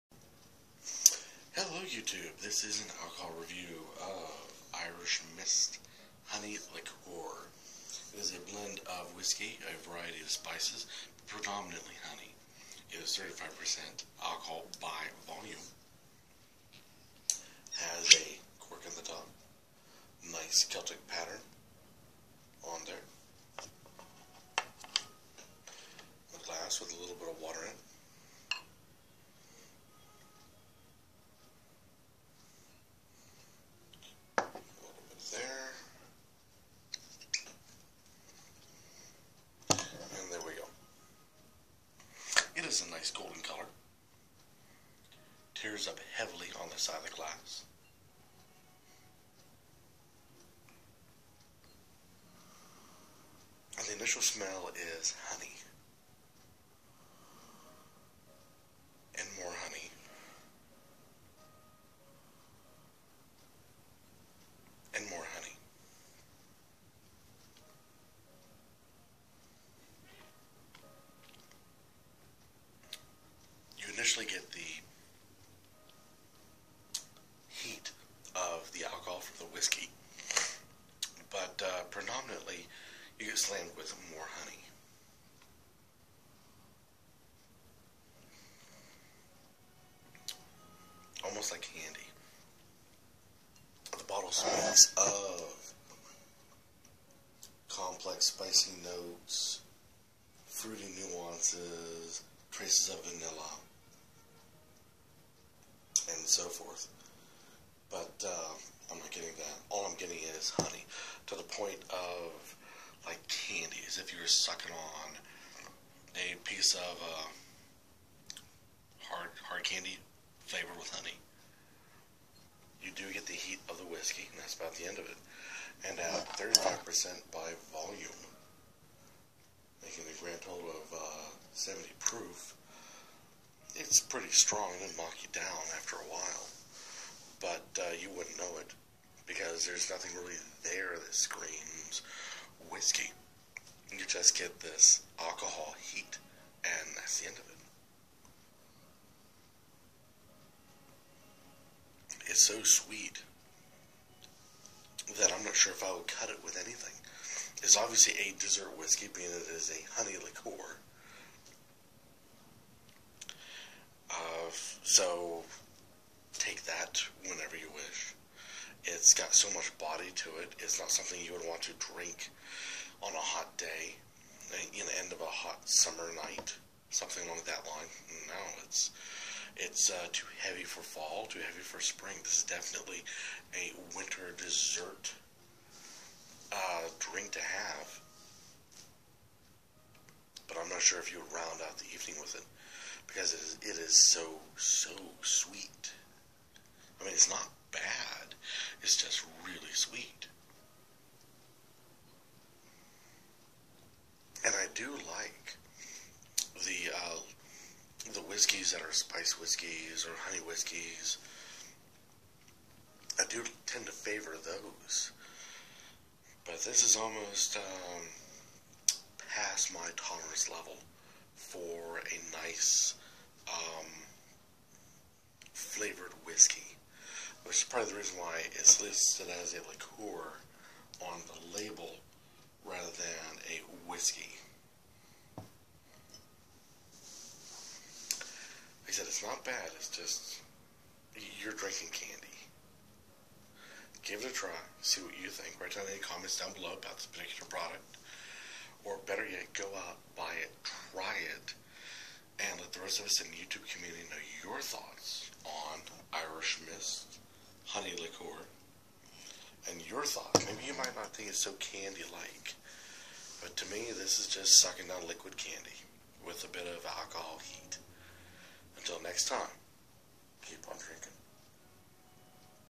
Irish Mist Honey Liqueur review
If you like it sweet,  I mean really sweet,  this is the one for you. It will knock you down at 70 proof, but you won't know it in time. Did I mention its sweet? Oh yeah,  it sweet.